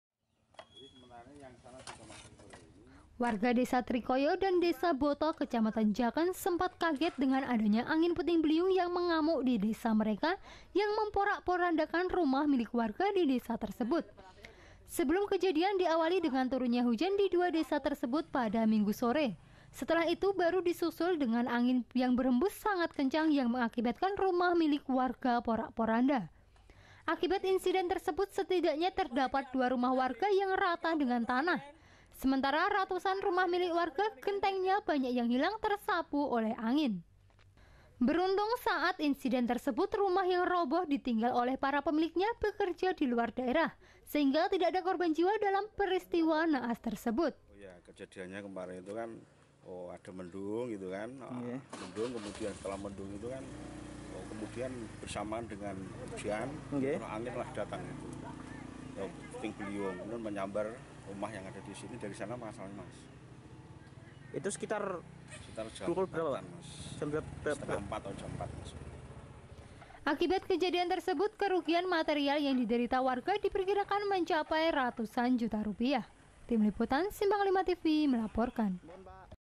PUTING BELIUNG, HANCURKAN DESA---BERITA 5 SIMPANG 5TV TGL 30 NOVEMBER 2015
LEAD
AKIBAT ANGIN YANG SANGAT KENCANG/ MENGAKIBATKAN RATUSAN RUMAH MILIK WARGA DI 2 DESA DI KECAMATAN JAKEN PORAK PORANDA// AKIBATNYA 2 RUMAH MILIK WARGA ROBOH/ DAN RATUSAN RUMAH LAINNYA GENTENGNYA HILANG TERSAPU
WARGA DESA TRIKOYO/ DAN DESA BOTO/ KECAMATAN JAKEN/ SEMPAT KAGET DENGAN ADANYA ANGIN PUTING BELIUNG YANG MENGAMUK DI DESA MEREKA/ YANG MEMPORAK-PORANDAKAN RUMAH MILIK WARGA DI DESA TERSEBUT//
SEBELUM KEJADIAN/ DIAWALI DENGAN TURUNNYA HUJAN DI 2 DESA TERSEBUT/ PADA MINGGU SORE// SETELAH ITU BARU DISUSUL DENGAN ANNGIN YANG BERHEMBUS SANGAT KENCANG/ YANG MENGAKIBATKAN RUMAH MILIK WARGA PORAK-PORANDA// AKIBAT INSIDEN TERSEBUT SETIDAKNYA TERDAPAT 2 RUMAH WARGA YANG RATA DENGAN TANAH/ SEMENTARA RATUSAN RUMAH MILIK WARGA GENTENGNYA BANYAK YANG HILANG TERSAPU OLEH ANGIN//
BERUNTUNG SAAT INSIDEN TERSEBUT/ RUMAH YANG ROBOH/ DITINGGAL OLEH PARA PEMILIKNYA BEKERJA DILUAR DAERAH/ SEHINGGA TIDAK ADA KORBAN JIWA DALAM PERISTIWA NA’AS TERSEBUT//
WWC : SANTOSA
AKIBAT KEJADIAN TERSEBUT/ KERUGIAN MATERIAL YANG DIDERITA WARGA DIPERKIRAKAN MENCAPAI HINGGA RATUSAN JUTA RUPIAH//
TIM LIPUTAN/ SIMPANG 5 TV/ MELAPORKAN//